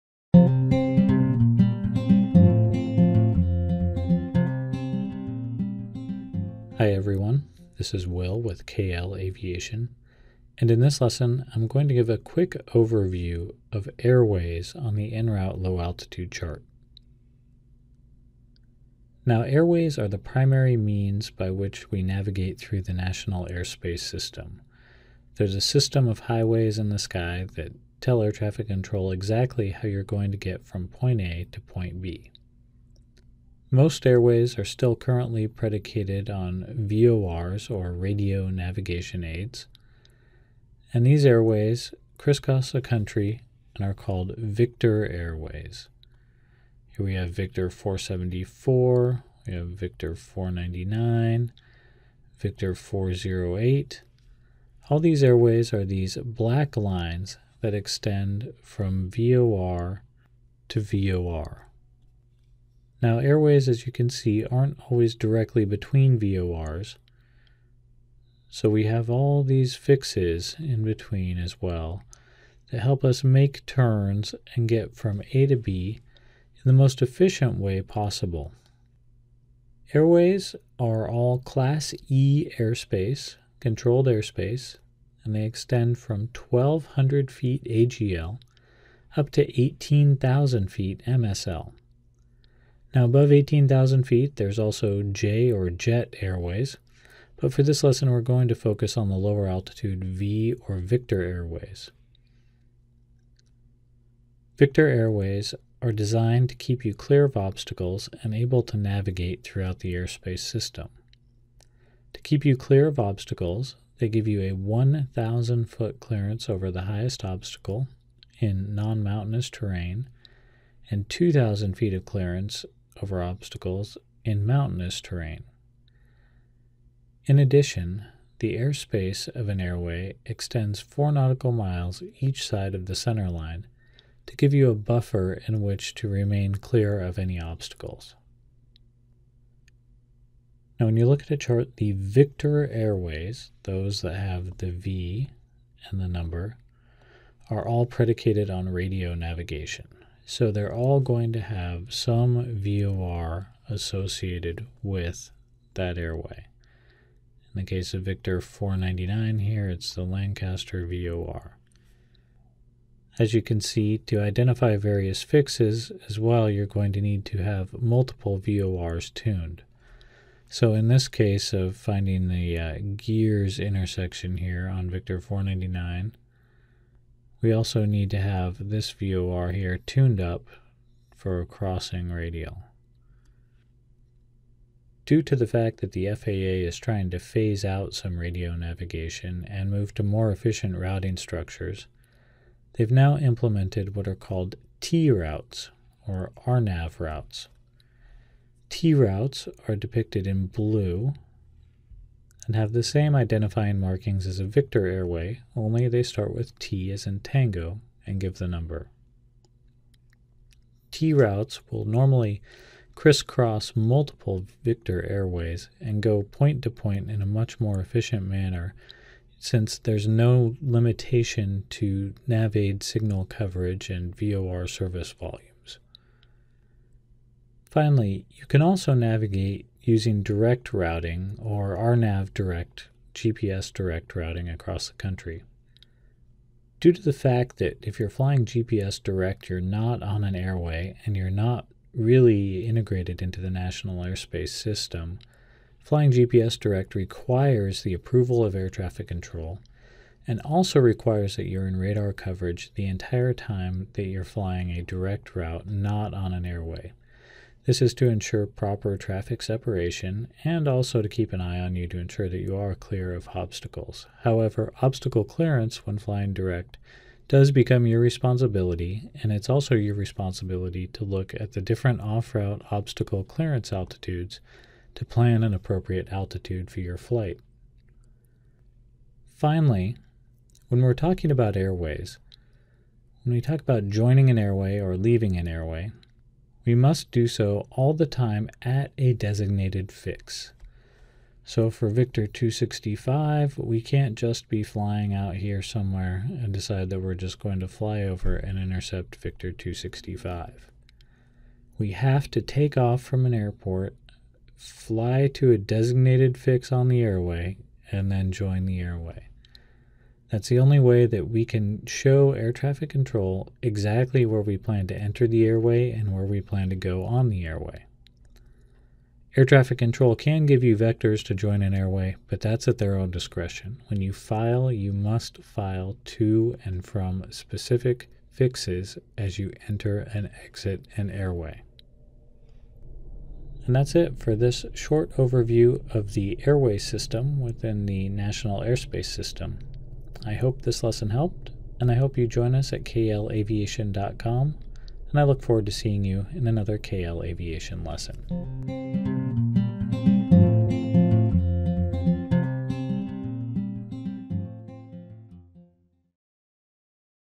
An Overview of Low Altitude Airways
This lesson is a quick overview of the airway system in the United States.

Airways are the "highways in the sky" that connect each point in the United States.  Airways tell air traffic controllers where we are going and how we are going to get there.  Without airways, all aircraft would be free to amble their way across the country on their own and in their own direction.

Most current airways are "V" or Victor airways. Victor airways are defined by VOR type navigational aids and rely entirely on ground based navigation.  This reliance on VORs means that victor airways have shorter segments that allow for proper signal reception between each station.  Victor airways are designed with a large number of "intersections", or points at which define intermediate segments of the route.  These intersections allow for continuous course monitoring and for bends within the airway.

Due to the FAA's desire to eliminate ground based navigation, new "T" routes are starting to be created.  T routes use RNAV/GPS for navigation and do not rely on any ground based navigational aids.  As a result, T routes can have much longer segments and go on a more direct path from point to point.  T routes are depicted just the same as Victor airways, except for the "T" preceding the number designation and the new blue color of the airway.

It is also possible to fly off route direct from point to point.  Direct flights require the approval of air traffic control and continuous radar contact.  This is to ensure traffic separation and obstacle clearance.  Obstacle clearance is assured by planning your flight to stay above the OROCA or Off Route Obstacle Clearance Altitude during your entire flight.

When filing a flight plan that involves airways, it is important to know that you can only join and exit an airway at a designated fix on the airway.  Air traffic control must know where you plan to enter and exit the highway system so they can provide proper separation and sequencing of traffic.  If traffic and workload permit, ATC can give you vectors to join an airway segment.

Hopefully this lesson helps!